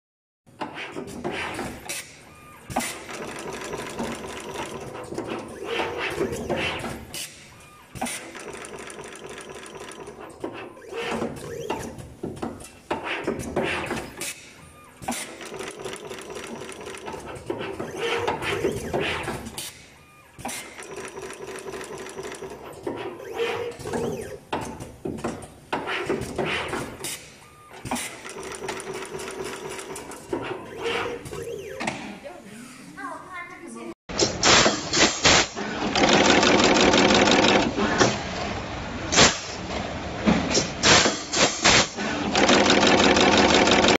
Stripping and Crimping Machine for Multi-core Round Cables
The multi-conductor sheathed cable terminal crimping machine can complete wire feeding, dividing and cutting, terminal crimping accurately and continuously.